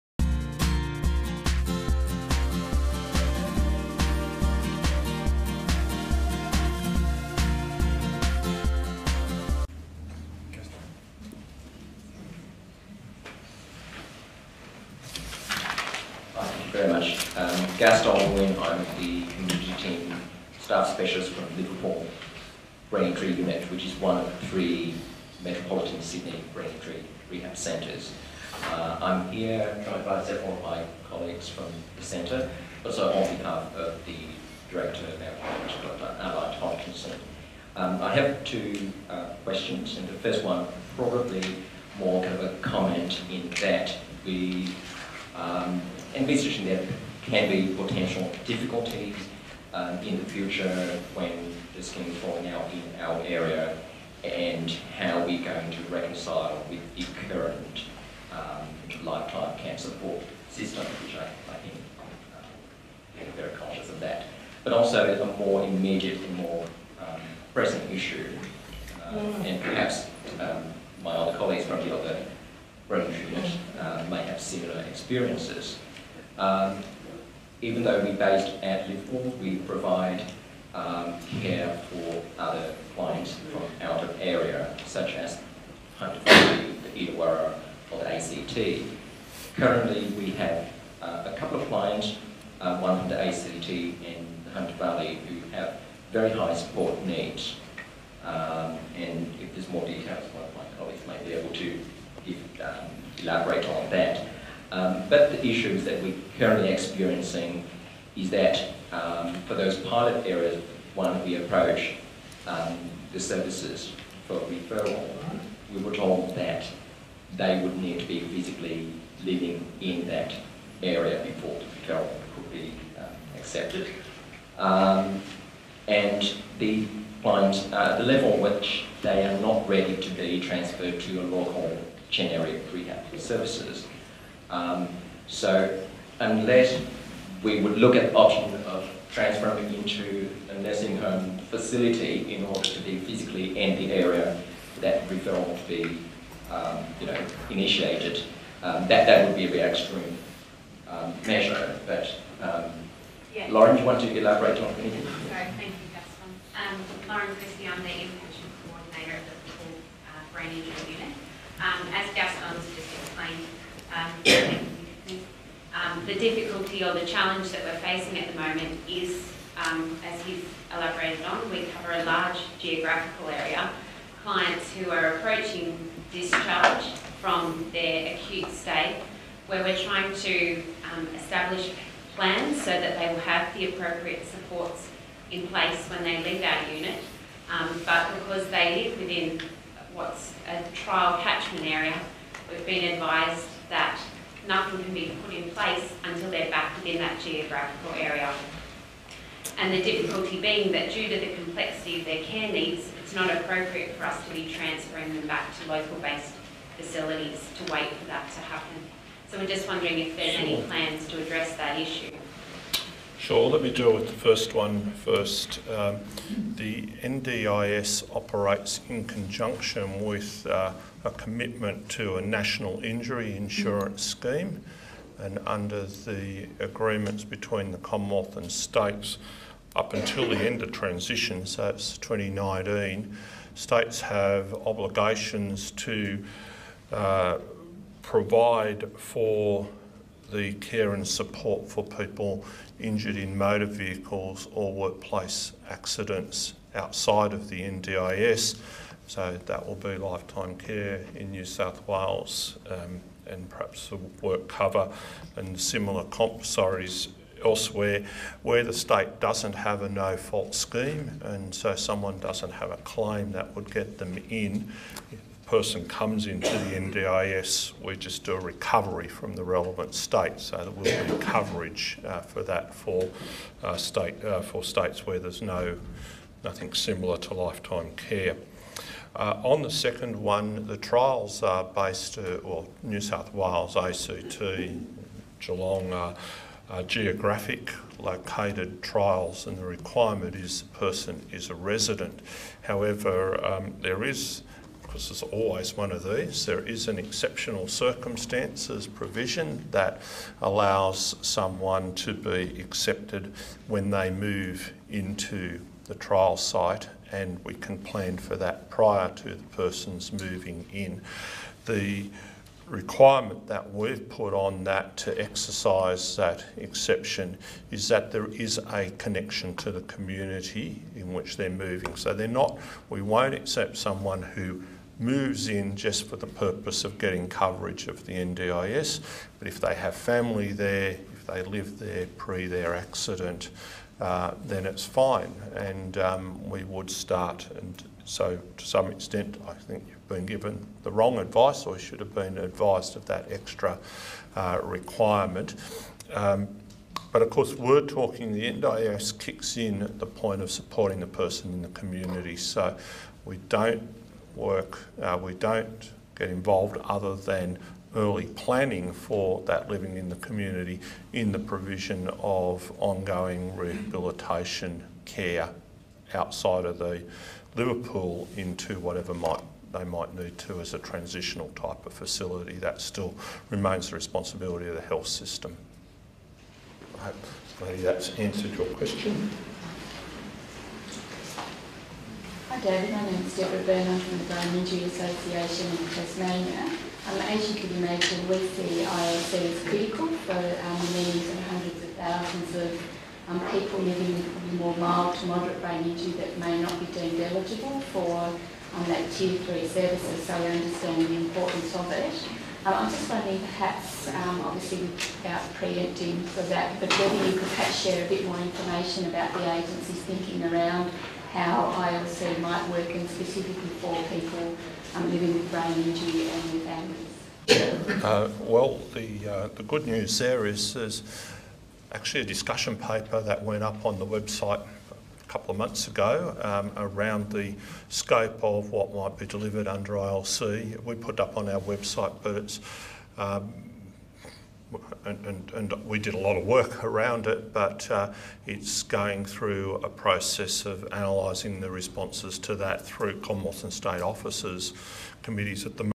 NDIS Forum 2015, David Bowen, Part 4
On 28 April 2015 Brain Injury Australia, in association with Sydney's
Royal Rehab, held a National Disability Insurance Scheme (NDIS) Forum with David Bowen, the CEO of the National Disability Insurance Agency.

Over100 consumers, family members, carers, clinicians, allied health  professionals, policy makers and researchers from New South Wales, Victoria, Tasmania, South and Western Australia - including  representatives from the NSW Lifetime Care and Support Authority,  Victoria's Transport Accident Commission and the universities of  Sydney, La Trobe, Flinders, and Macquarie - got a rare opportunity to raise questions about the design and implementation of the NDIS for the over 700,000 Australians with a brain injury.